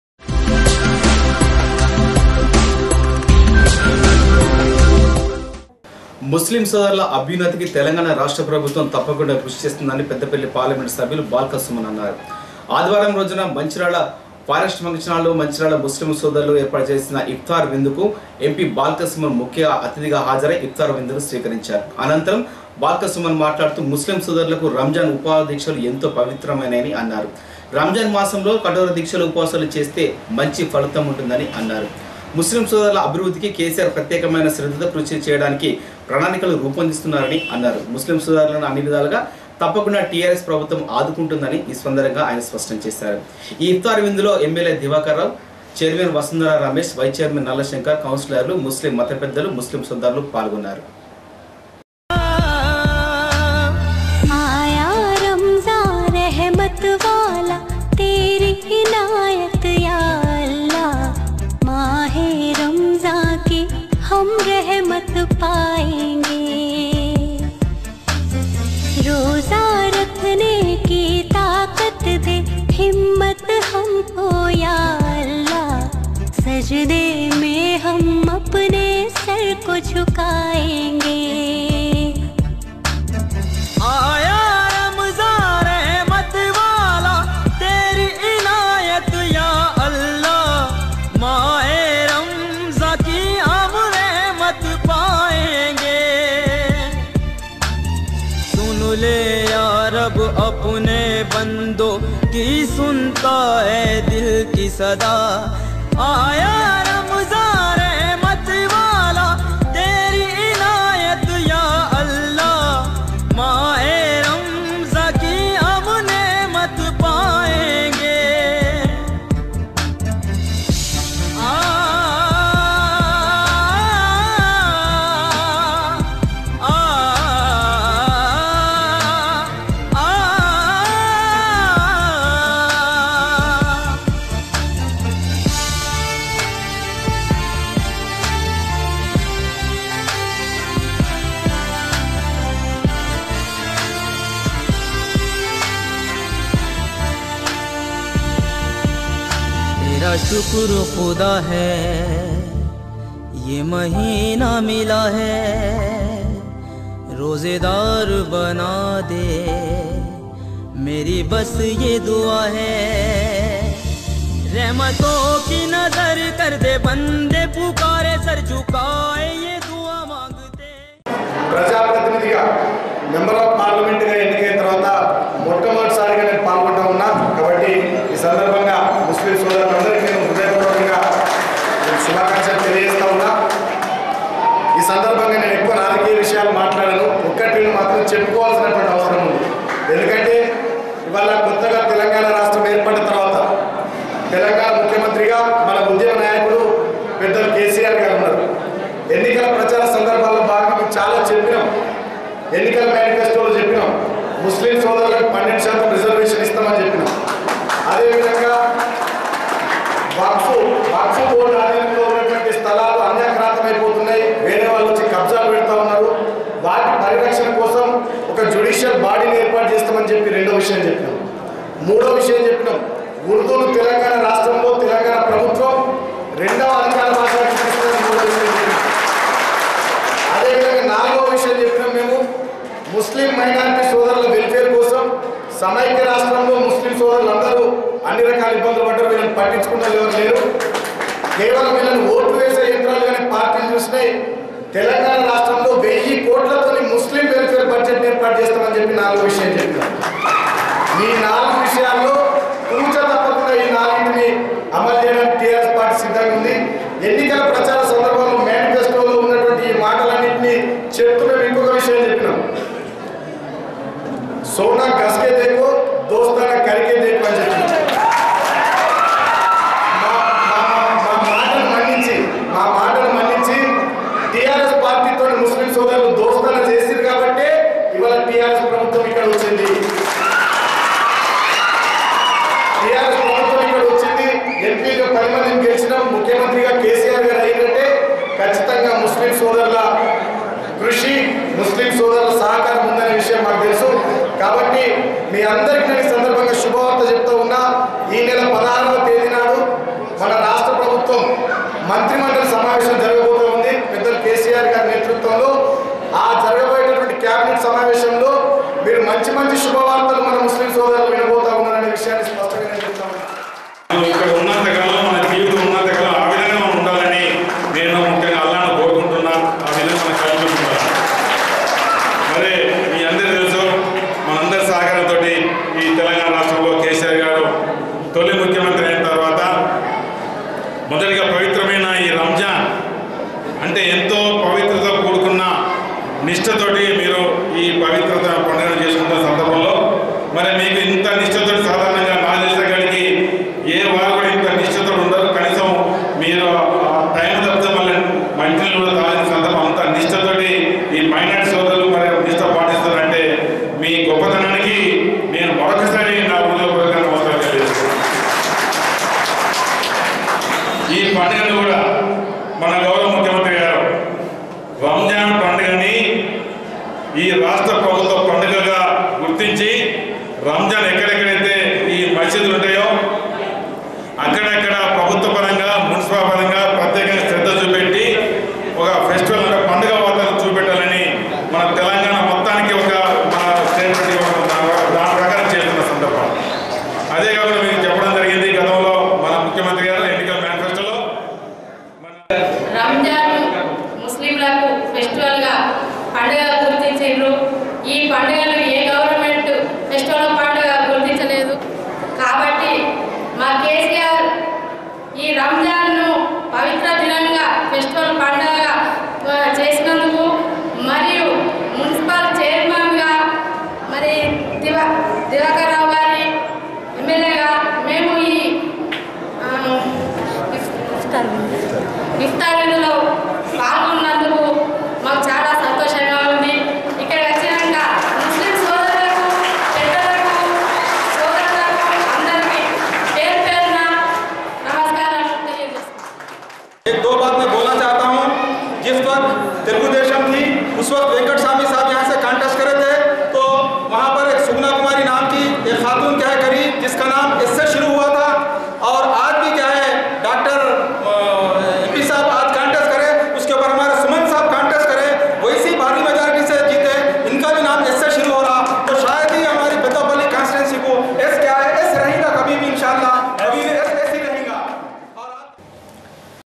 13-07-2014 Iftar party - Mancherial
Iftar party - Mancherial
Special Chief Guest MP Balks Suman Garu, Nadipeeli Diwakr Rao garu, Municipal Chair Person Vasundhar garu.